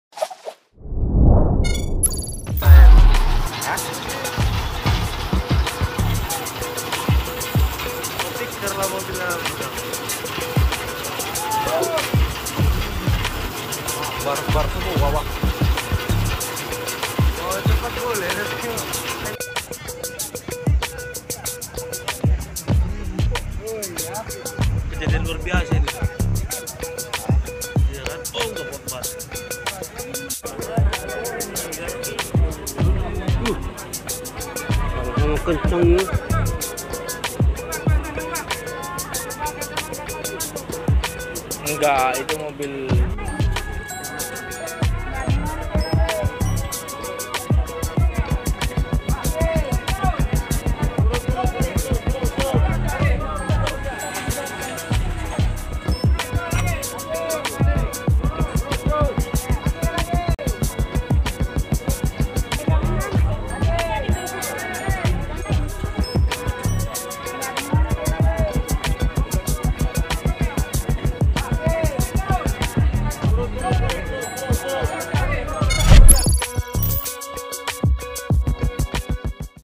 Ringsek Parah! Ini Penampakan Kecelakaan 2 Bus di Jalinsum Sumut
Senin (20/6) dini hari, kecelakaan maut antara Bus PMH dan Bus PMS terjadi di Jalan Lintas Sumatera (Jalinsum), tepatnya di Desa Asam Jawa, Kecamatan Torgamba, Labuhanbatu Selatan (Labusel), Sumatera Utara. Korban tewas akibat peristiwa ini mencapai 6 orang.